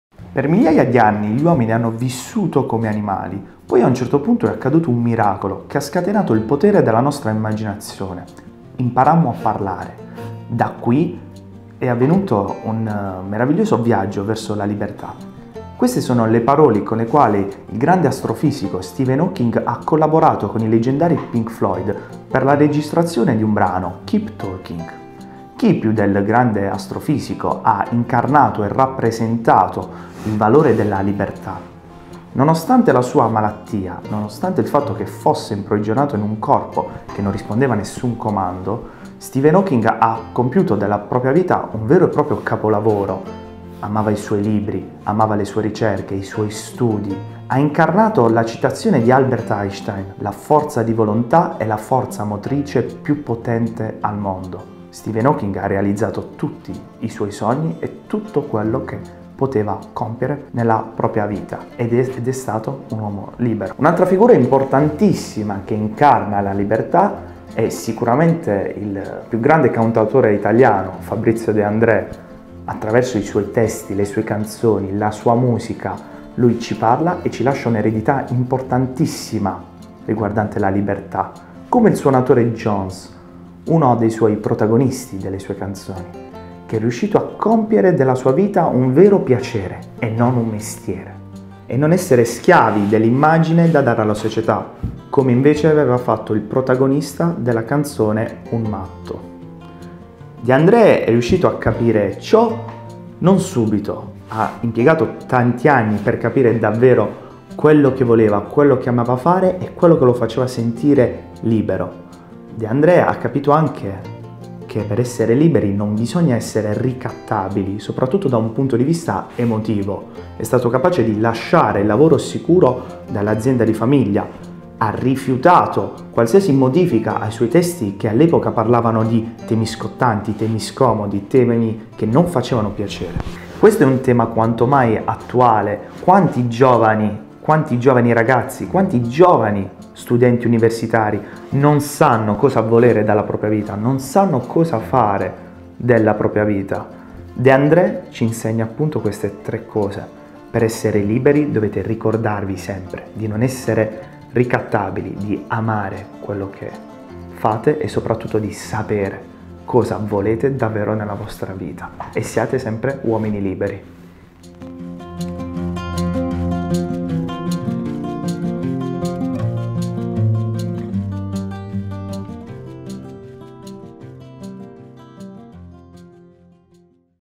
Libertà: da De Andrè a S. Hawking
Quando ci sentiamo davvero "liberi"? che cosa rappresenta per noi la libertà? che cosa accomuna De Andrè a S. Hawking? quali sono i segreti per raggiungere la nostra libertà?